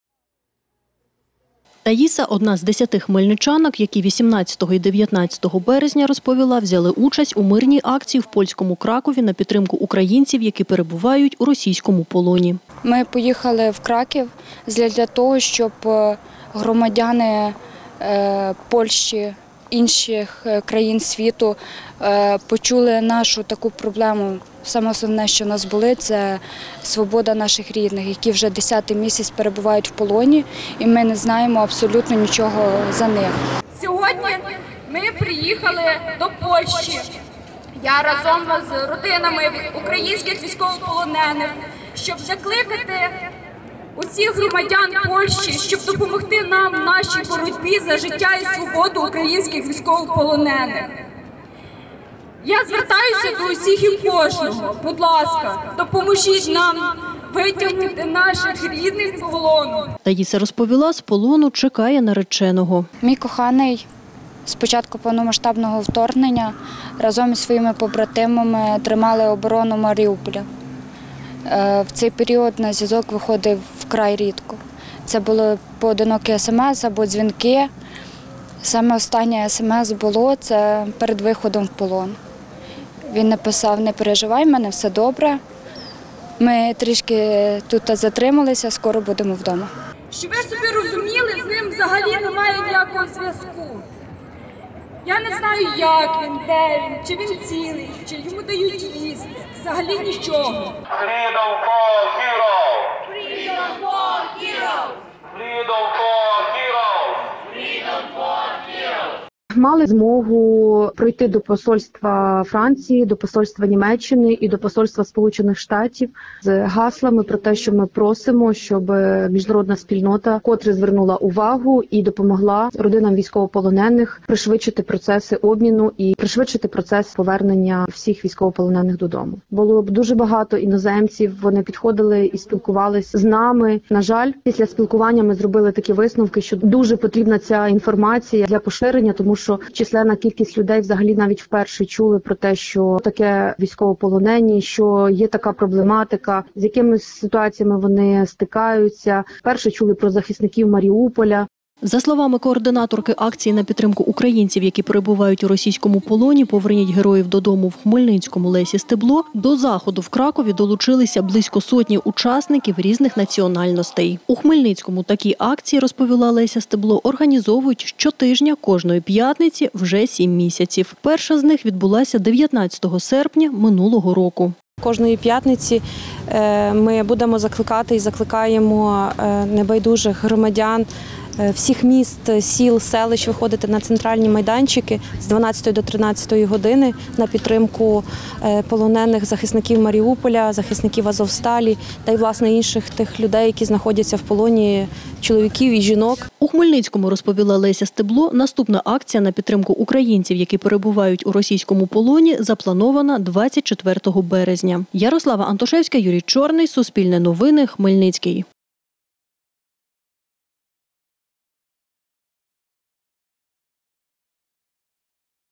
За кордоном не всі знають про наших військовополонених. Хмельничани - про акцію у Польщі
Звертатимуться до усього світу, аби повернути рідних. За словами Таїси, нареченої українського воїна, який перебуває в російському полоні, з такими планами повернулися хмельничани з Кракова. Туди вони їздили на мирну акцію і з’ясували, що про ситуацію із нашими військовополоненими пересічним полякам мало що відомо. Деталі Суспільне дізнавалося.